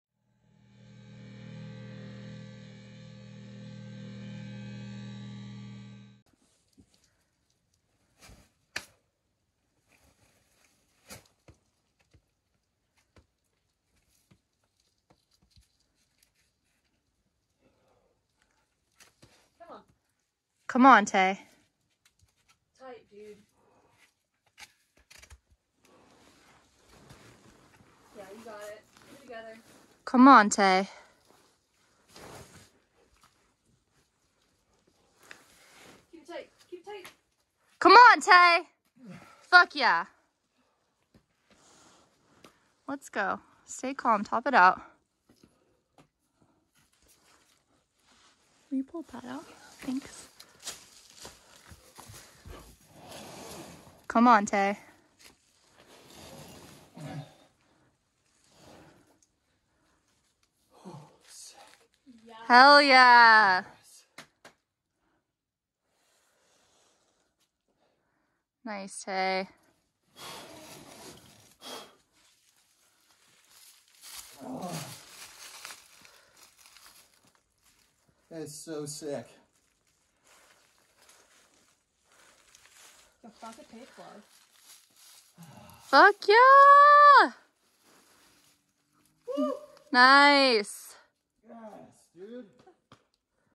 UNCUT: Taylor McNeill - Sailor's Delight (V14) First Ascent
Taylor establishing a new one in the back woods of North Carolina